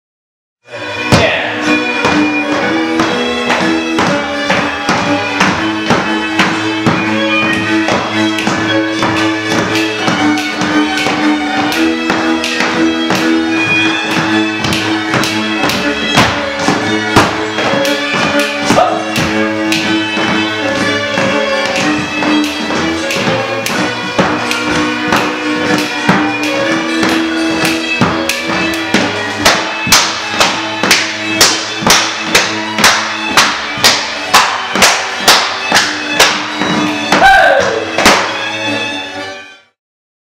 Tisza - Szatmári class #4 review (2013) - lead around from open position (with music)
2013, Feb. 21 - Kensington, MD. Review of figure (and variations) where the man leads the woman around him from the rida position.